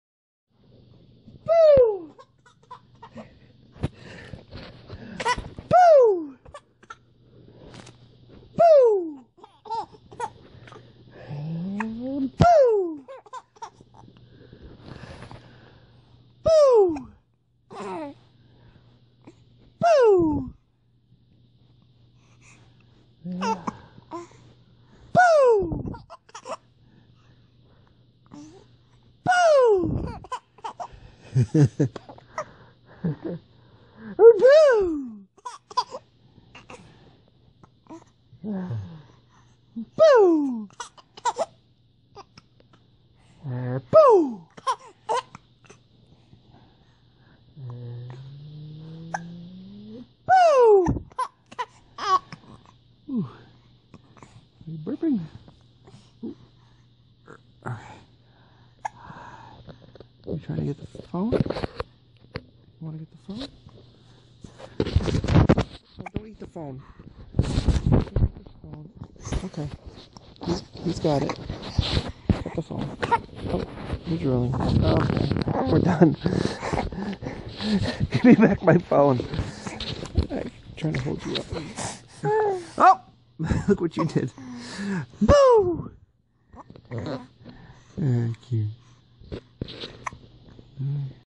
854 - Laughing Baby (Boo!)
Kieran laughs real well now at about 5 months! :) ...